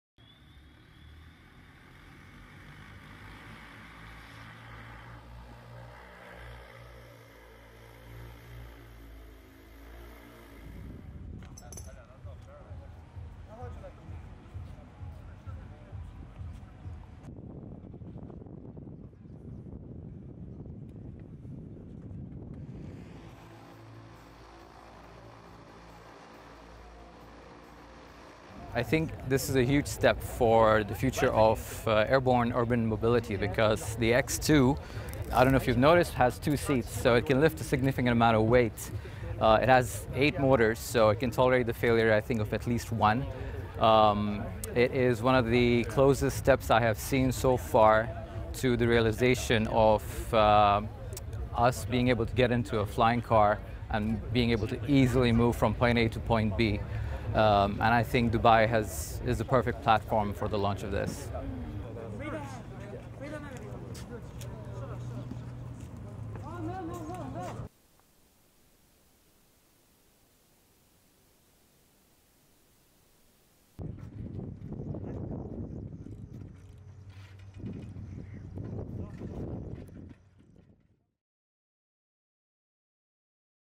Flying car takes first public flight in Dubai
The two-seater XPeng Aeroht electric VTOL X2 wowed crowds at Gitex